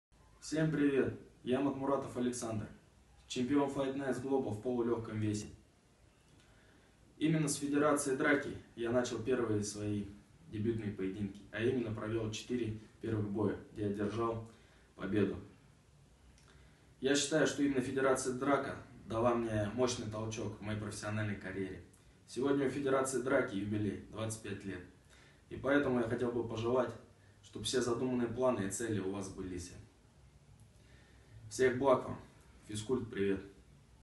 Александр Матмуратов. Поздравление к юбилею "ДРАКА"
Чемпион Fight Nights Global в полулегком весе Александр Матмуратов. Поздравление к 25-летнему юбилею Федерации боевых искусств "ДРАКА"